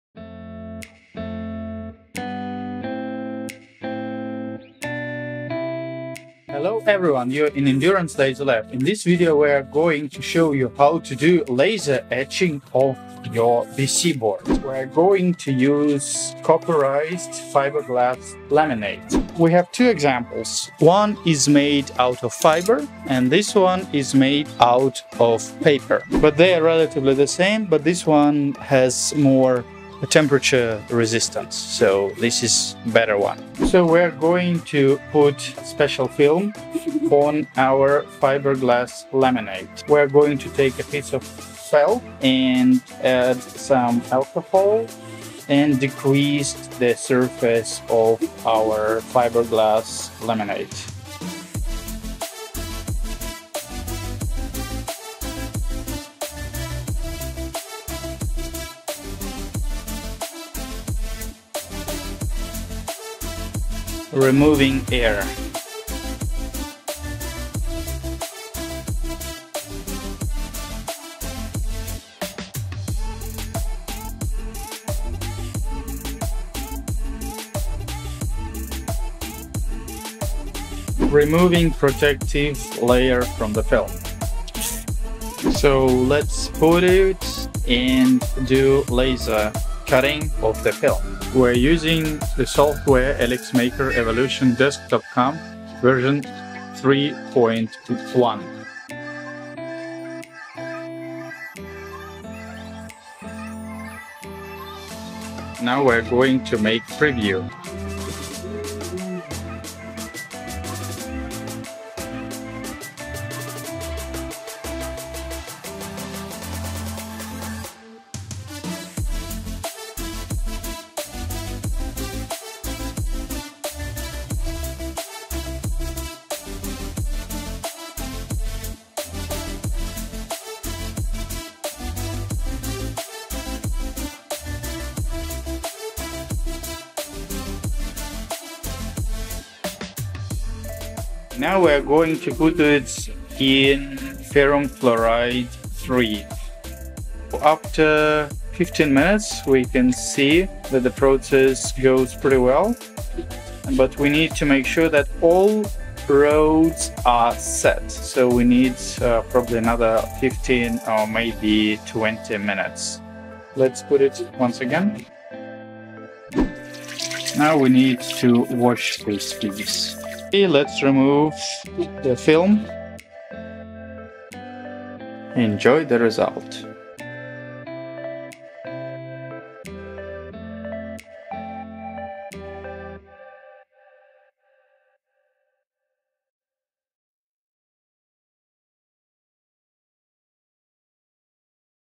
Laser etching of a copporized fiberglass laminate using 10 watt+ Endurance laser
How to make your own PCB using a copporized fiberglass laminate and powerful Endurance 10 watt+ laser attachment